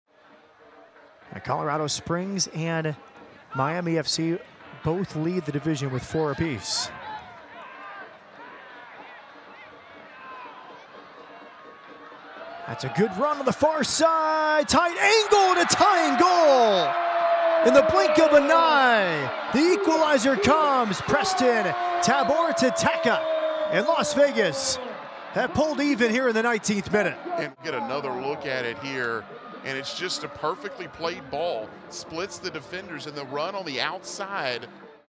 Tabort Preston with a Goal vs. San Antonio FC
Tabort Preston (Las Vegas Lights FC) with a Goal vs. San Antonio FC, 05/07/2023